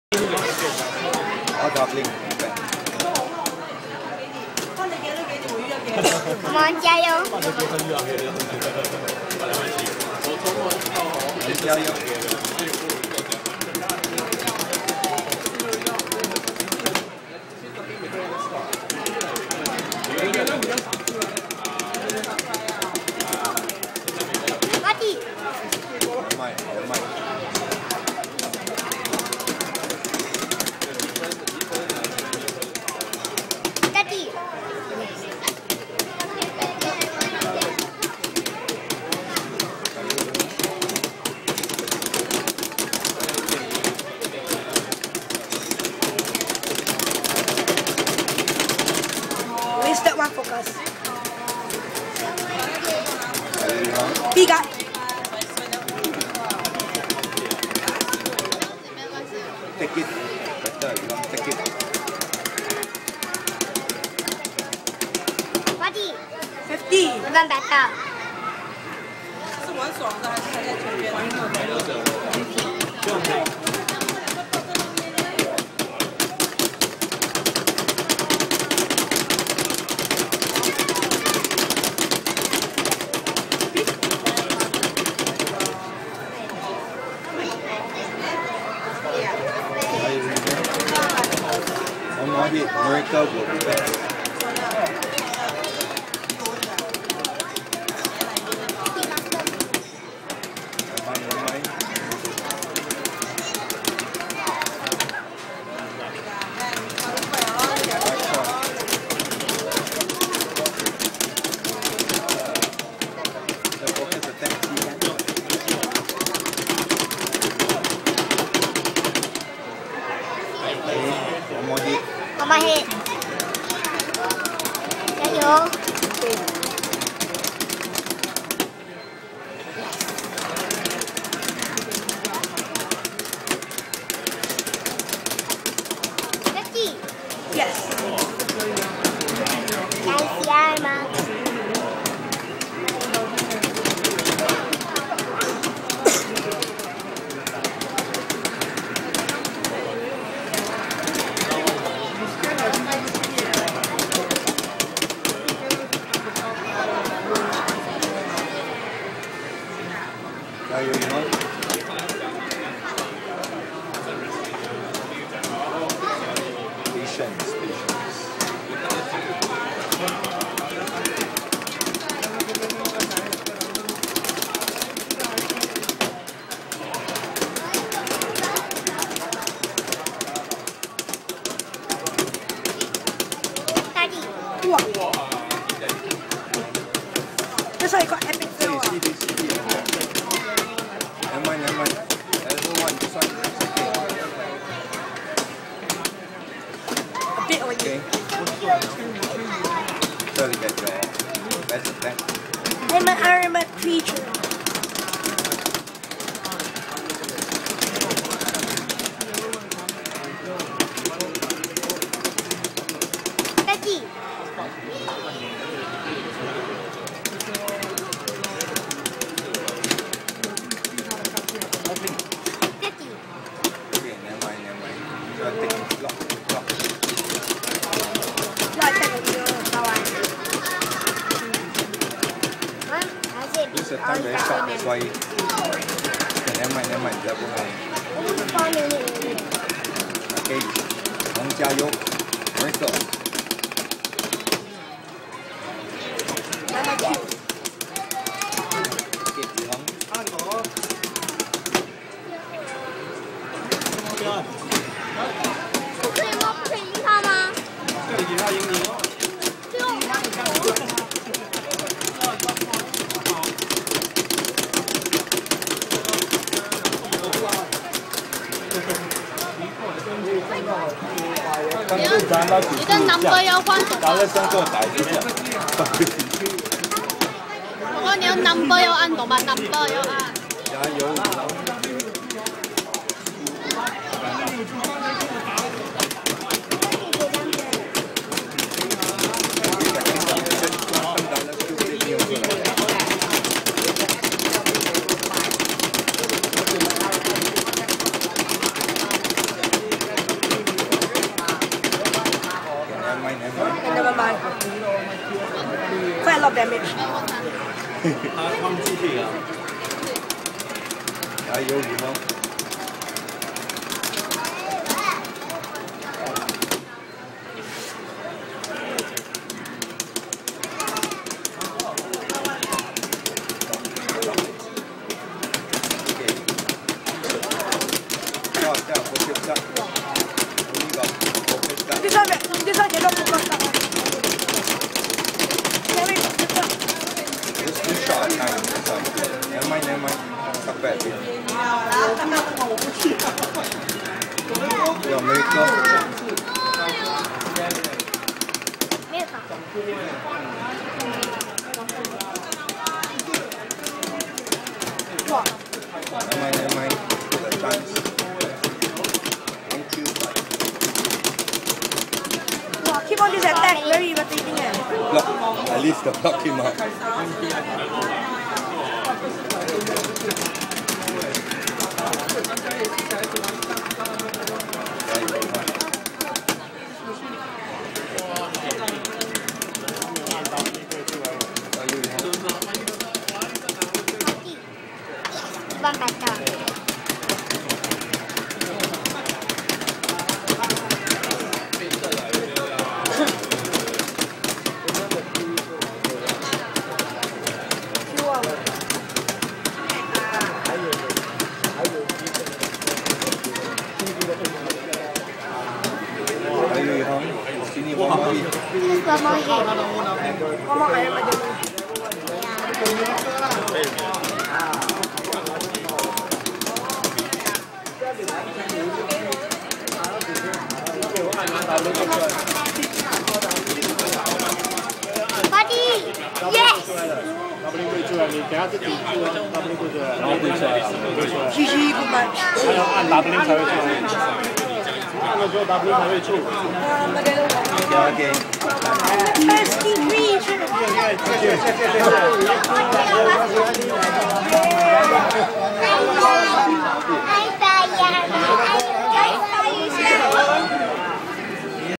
Indian Rhino vs Oorlog (Final)
This is the final match in the bronze special tournament at United Square Shopping Mall on 8 June 2013 (3pm).